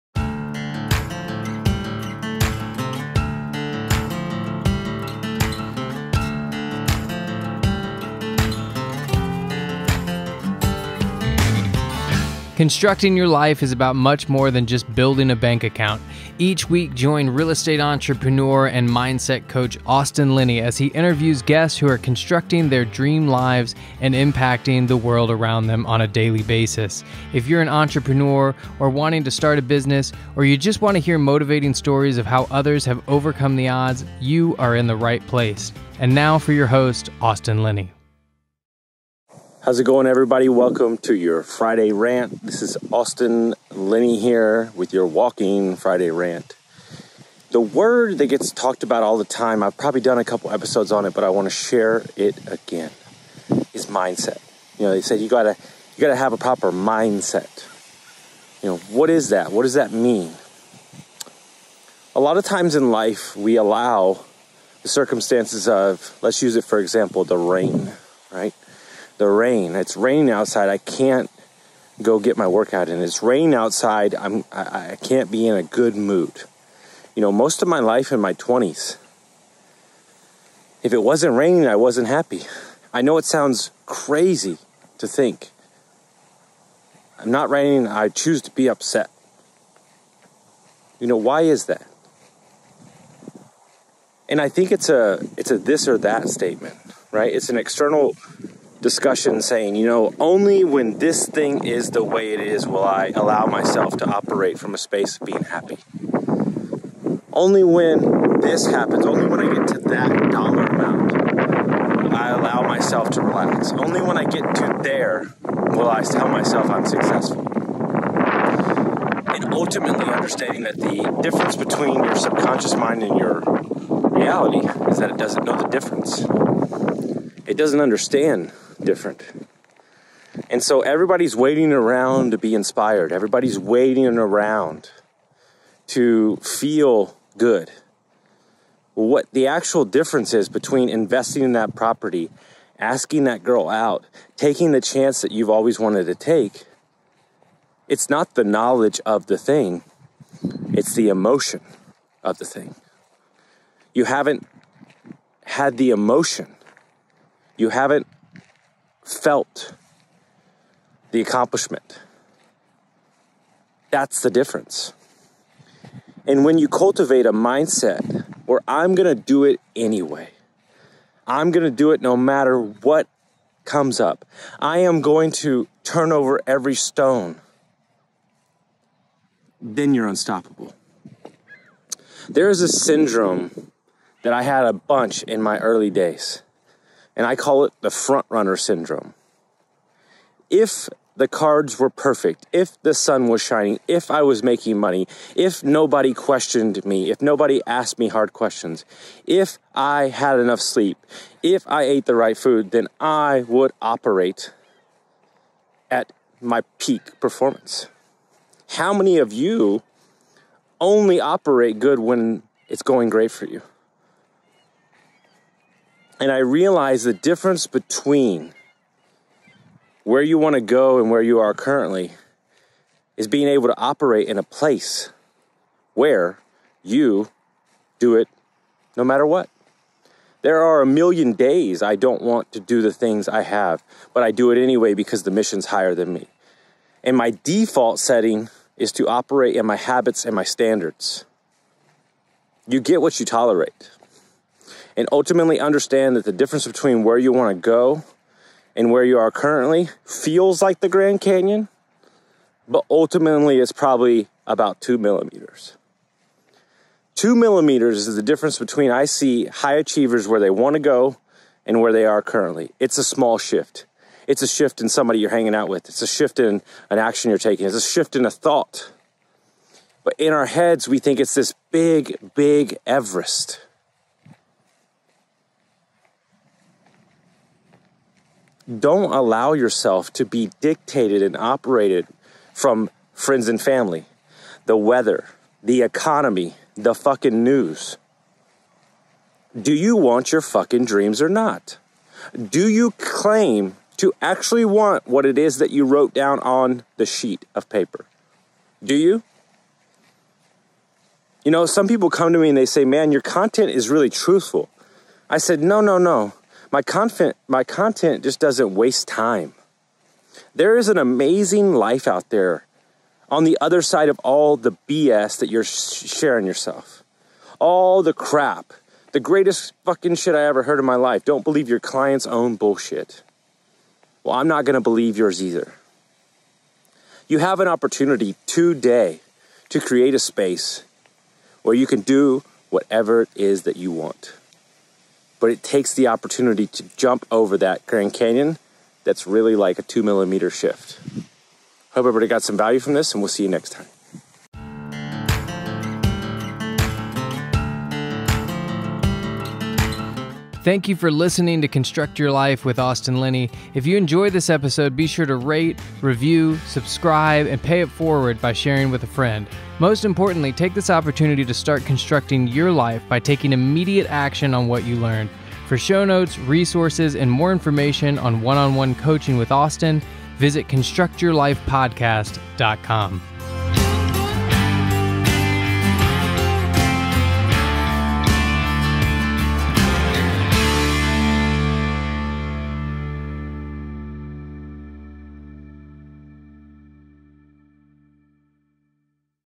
What is the Peak Mindset? | Friday Rant | Construct your life #408
In this episode of the Friday Rant, Austin Linney cracks the code on "Peak performance Mindset". In fact, he will tell you exactly which elements is to take in consideration when it comes to reaching your peak state. What if it only depends on you? What if outside elements had nothing to do with your state of being?

to get your free 3 steps to Construct your life